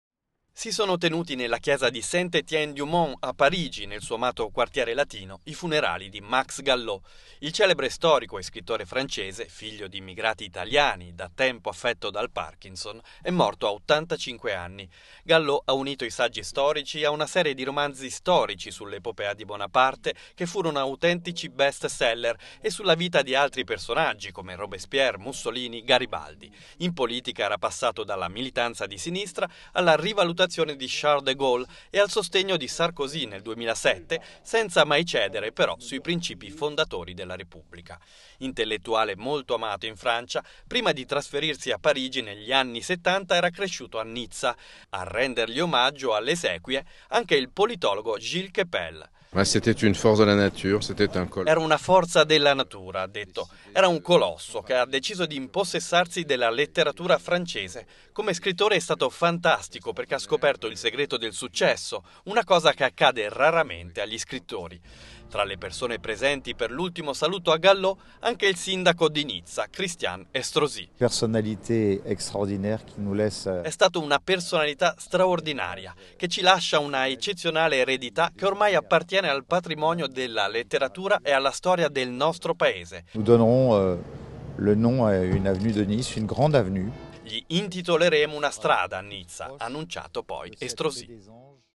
A Parigi l'ultimo saluto allo storico e scrittore Max Gallo
Parigi (askanews) - Si sono tenuti nella chiesa di Saint Etienne du Mont a Parigi, nel suo amato quartiere latino, i funerali di Max Gallo. Il celebre storico e scrittore francese, figlio di immigrati italiani, da tempo affetto dal Parkinson, è morto a 85 anni.
Gallo ha unito i saggi storici a una serie di romanzi storici sull'epopea di Bonaparte, che furono autentici bestseller, e sulla vita di altri personaggi come Robespierre, Mussolini, Garibaldi. In politica era passato dalla militanza di sinistra, alla rivalutazione di Charles de Gaulle e al sostegno di Sarkozy nel 2007, senza mai cedere sui principi fondatori della Repubblica.
Intellettuale molto amato in Francia, prima di trasferirsi a Parigi negli anni settanta, era cresciuto a Nizza. A rendegli omaggio alle esequie, anche il politologo Gilles Kepel: "Era una forza della natura - ha detto - era un colosso che ha deciso di impossessarsi della letteratura francese. Come scrittore è stato fantastico perché ha scoperto il segreto del successo, una cosa che accade raramente agli scrittori".
Tra le persone presenti per l'ultimo saluto a Gallo,anche il sindaco della città della Costa Azzurra, Christian Estrosi: "E' stato una personalità straordinaria, che ci lascia una eccezionale eredità che ormai appartiene al patrimonio della letteratura e alla storia del nostro Paese". "Gli intitoleremo una strada a Nizza", ha annunciato poi Estrosi.